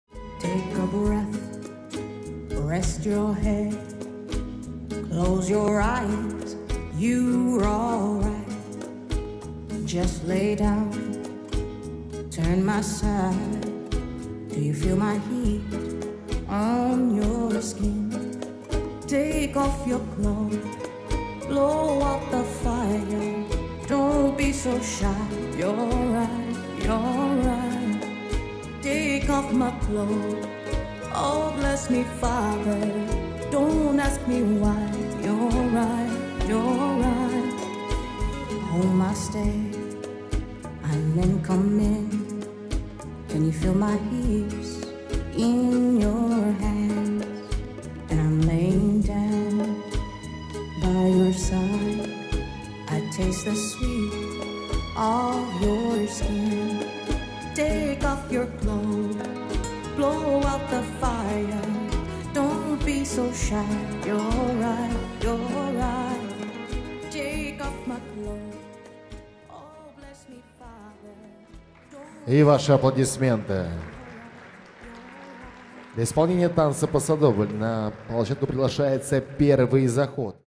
Maksim Maksimov - Margarita Dzyuba | R1 Rumba | North Star 2020
North Star 2020
Amateur Latin, Round 1, Rumba
Maksim Maksimov - Margarita Dzyuba